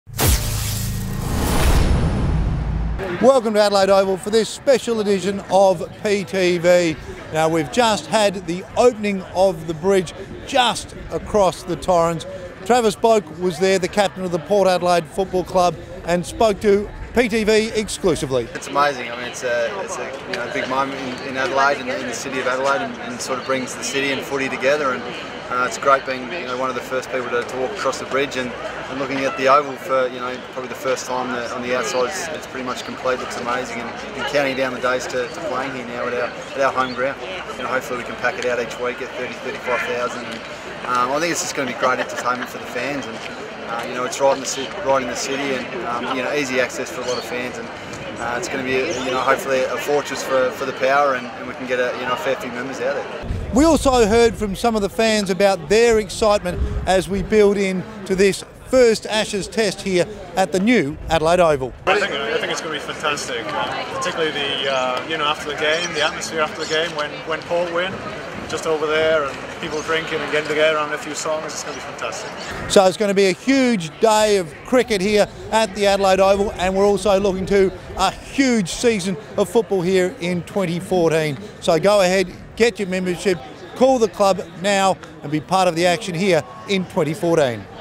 PTV: Adelaide Oval bridge opening with Travis Boak
PTV caught up with Travis Boak at the opening of the footbridge across the River Torrens to get his thoughts on our new home ground.